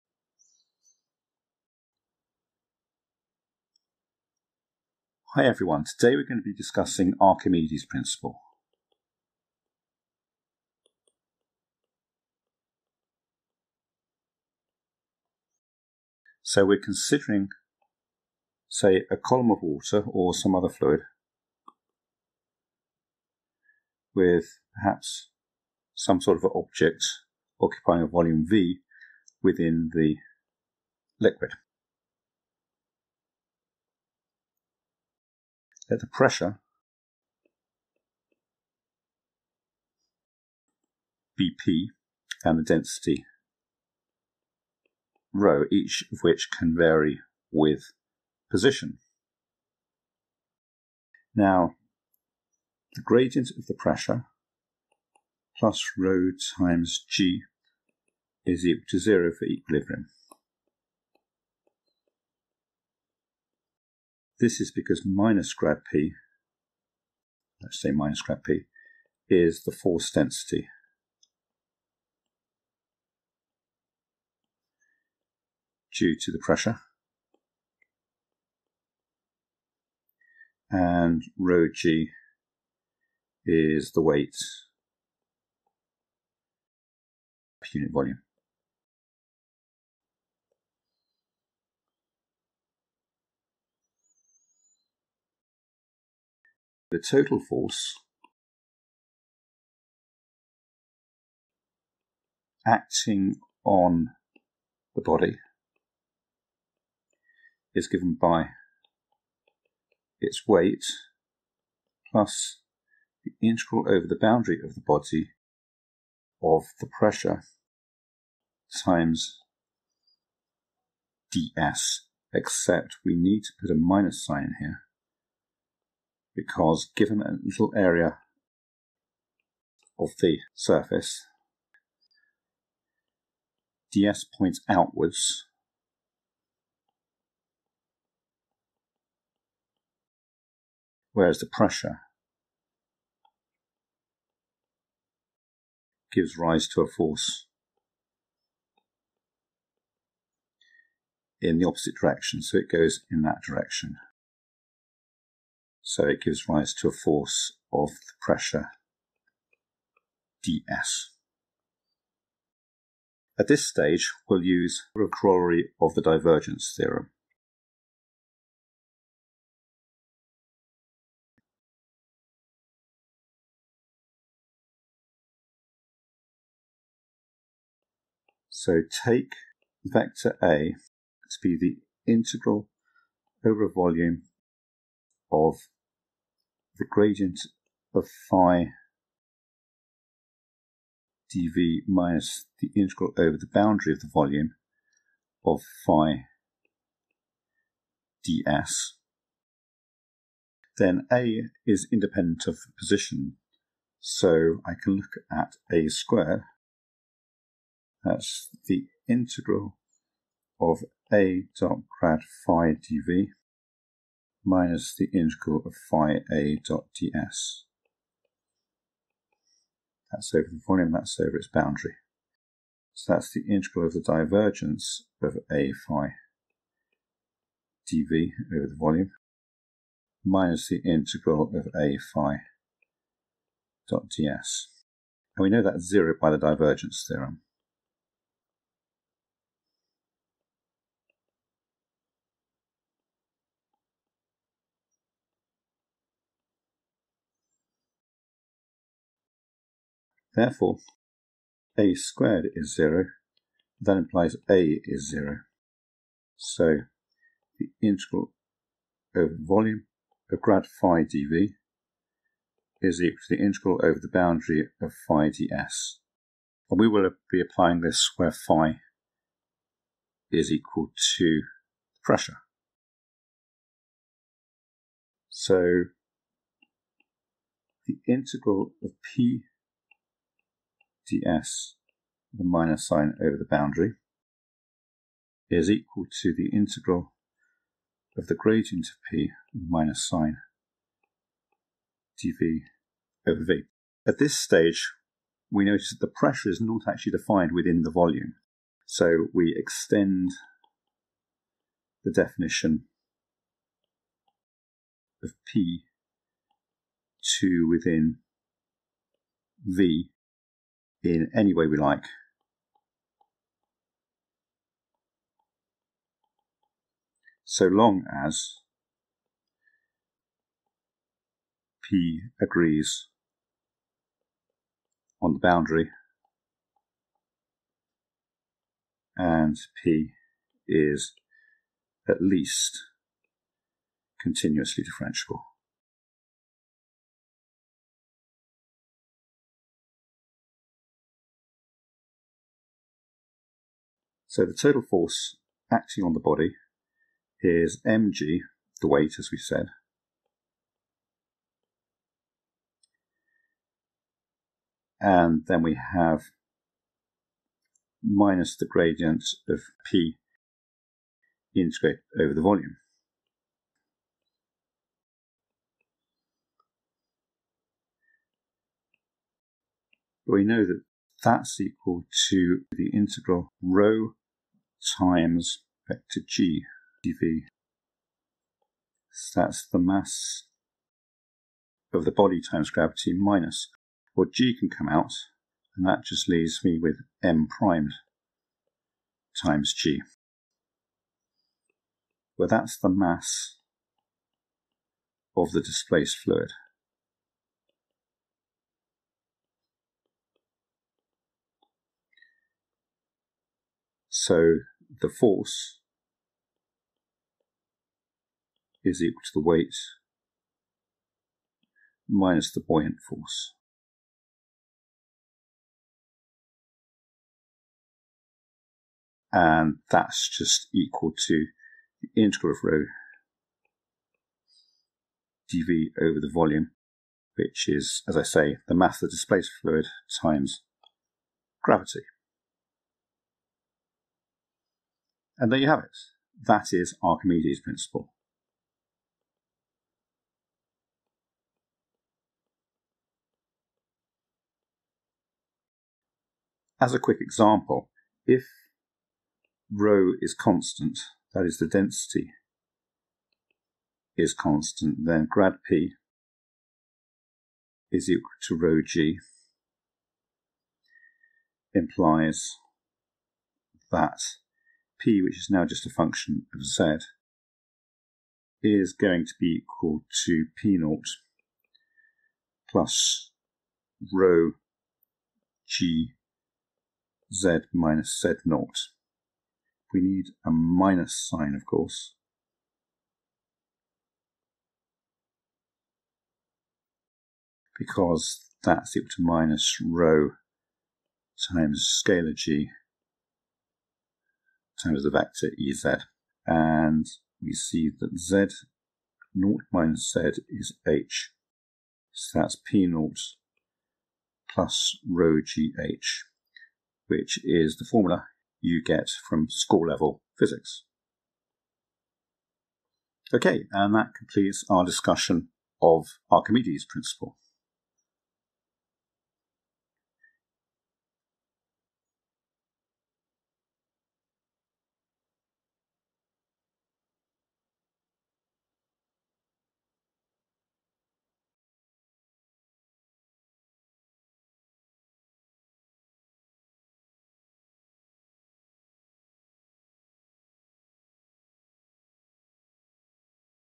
Archimedes Principle
In this video, we derive Archimedes Principle of Upthrust. By integrating the pressure over the surface of a body submerged in a static liquid we compute the buoyant force in terms of the weight of the liquid displaced.